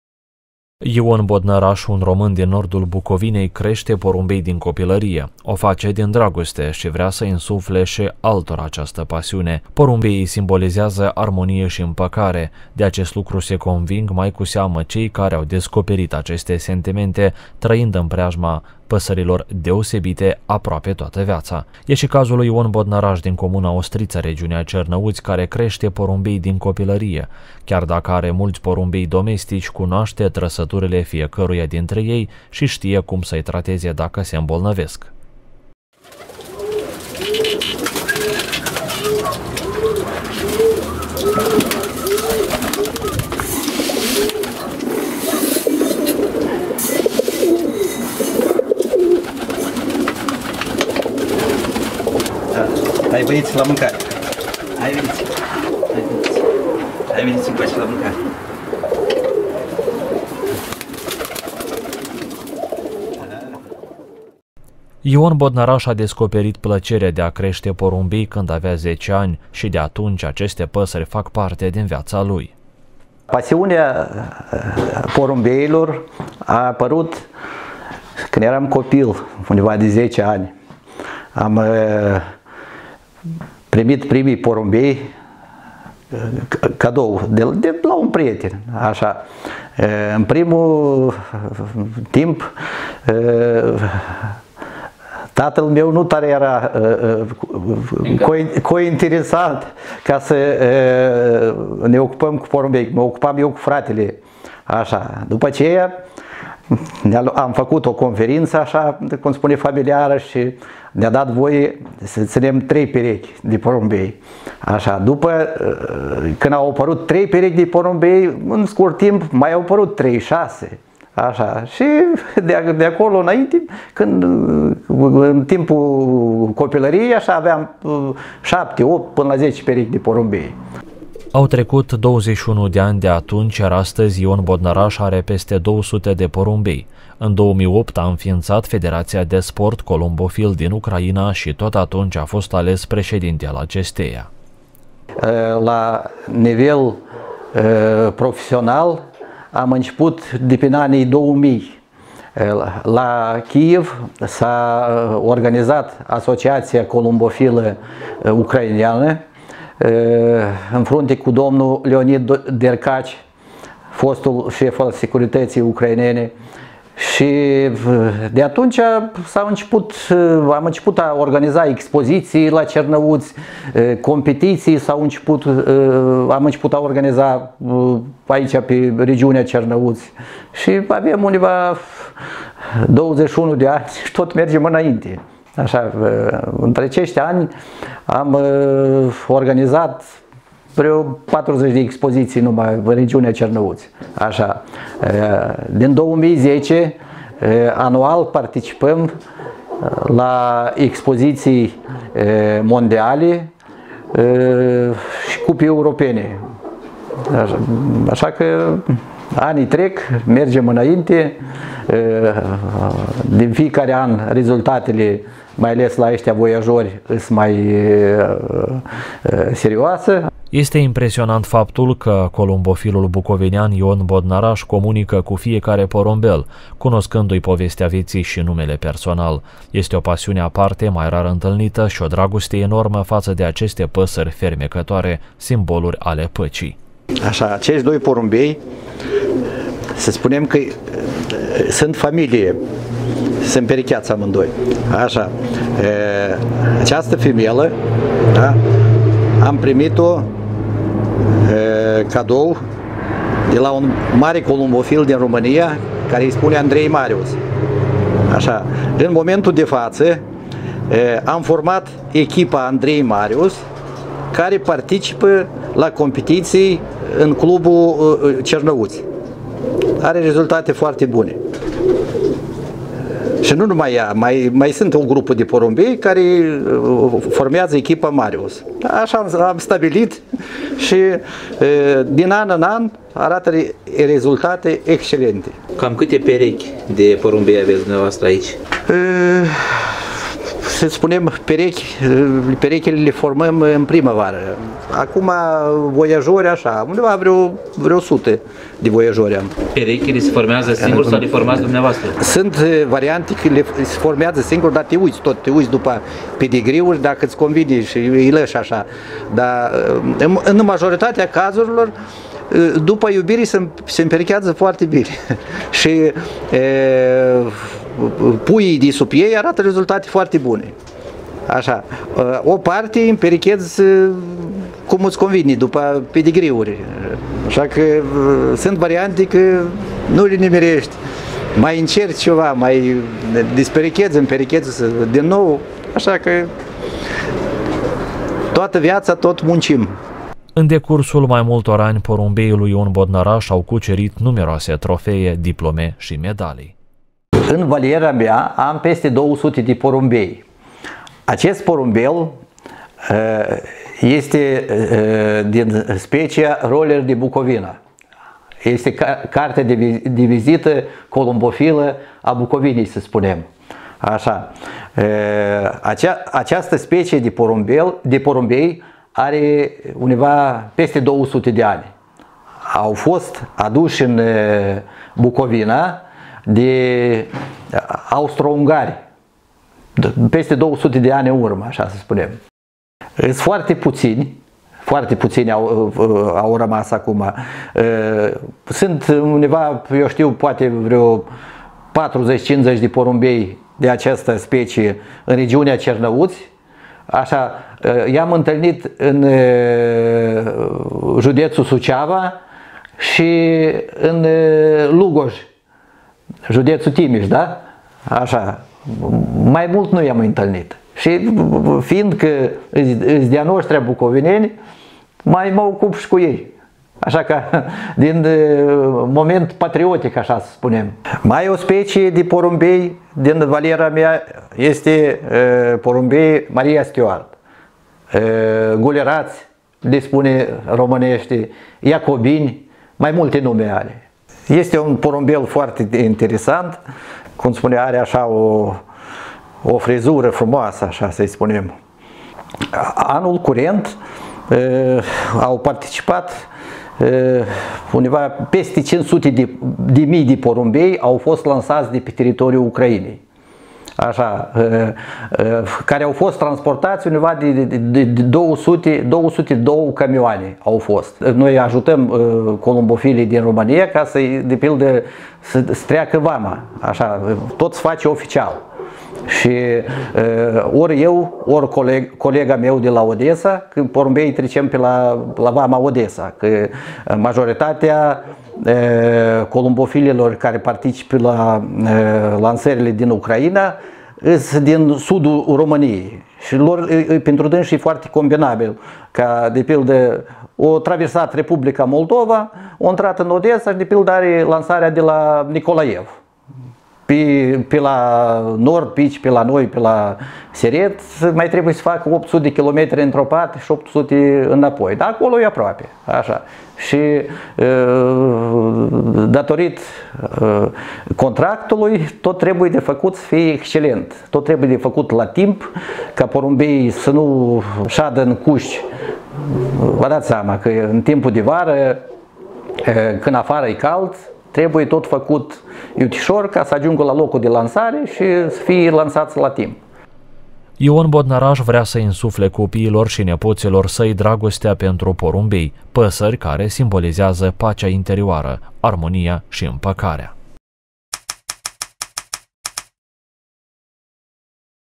BucPress TV: Un român din Bucovina, președinte al Federației de sport columbofil din Ucraina
BucPress TV Cernăuți: Ion Bodnăraș – un român din nordul Bucovinei, președinte al Federației de sport columbofil din Ucraina